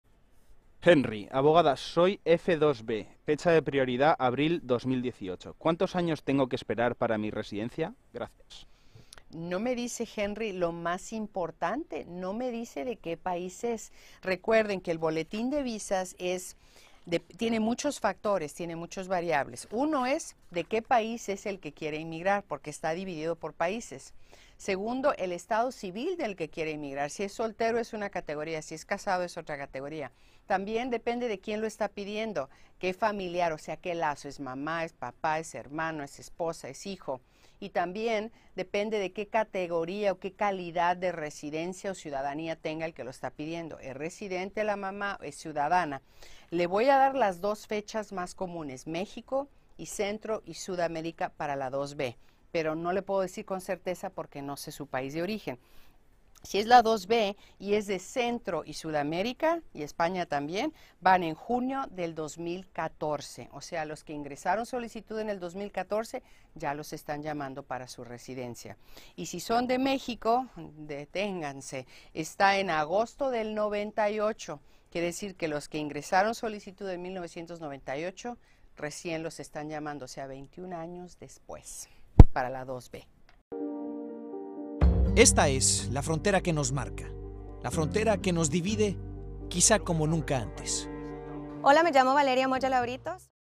¿Cuántos años hay que esperar para la residencia con F2B? | Noticias Telemundo
Video oficial de Noticias Telemundo. Una persona con F2B tiene diferentes fechas dependiendo el país de donde venga y la categoría donde pertenezca.